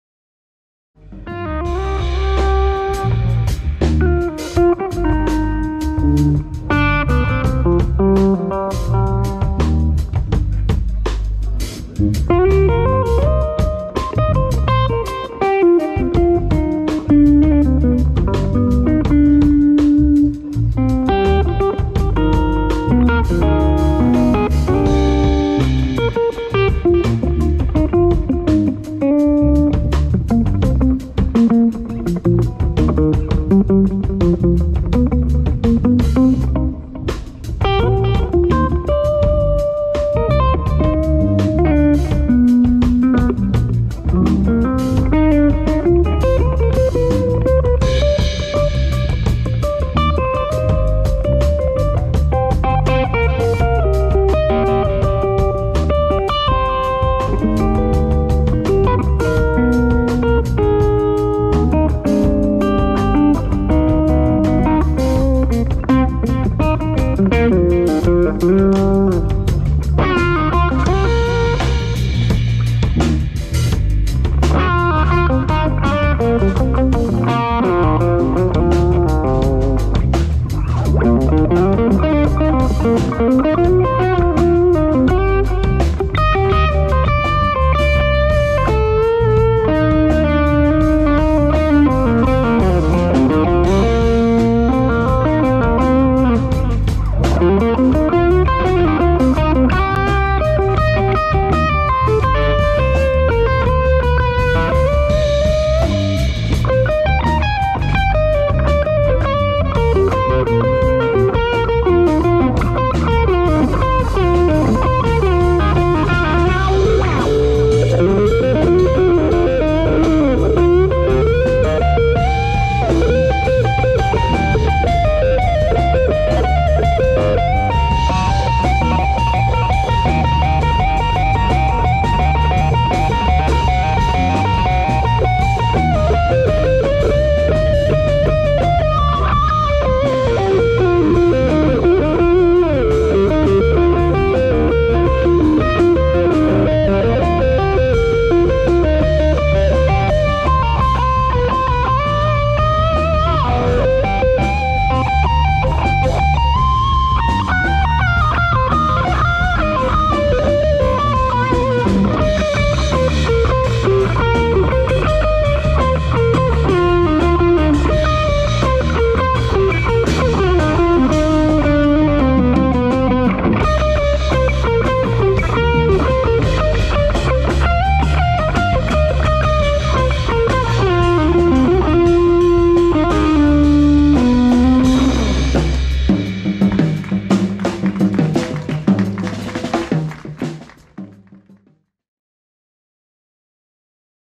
Jazz - Rock Guitar Solo, International Jazz Day
Thursday Reading Club Live at International Jazz Day - NISVILLE!

Recorded by GoPro Hero 9...